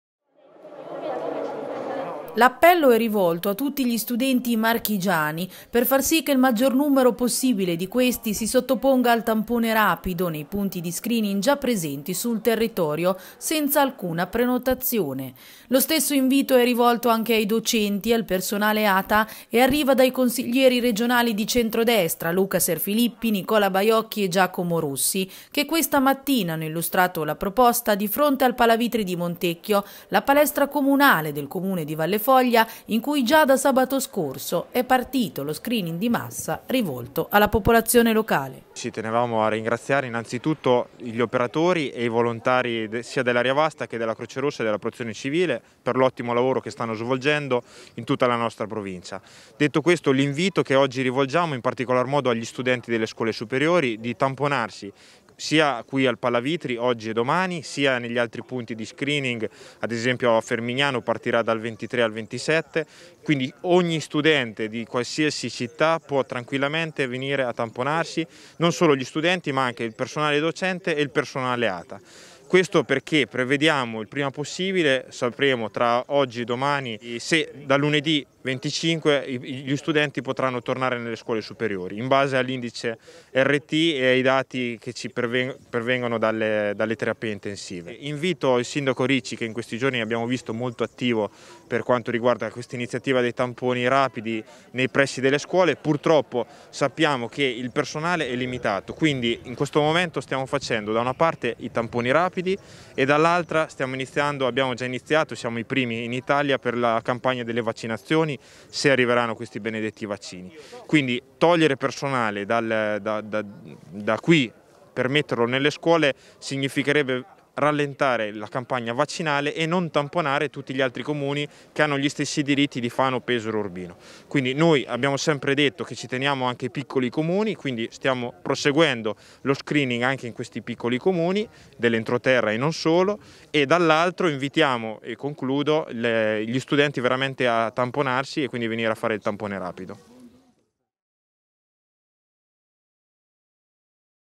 Serfilippi: "Tampone agli studenti"
I consiglieri hanno lanciato un appello a fare il test, messo a disposizione degli studenti, che torneranno a scuola dalla prossima settimana.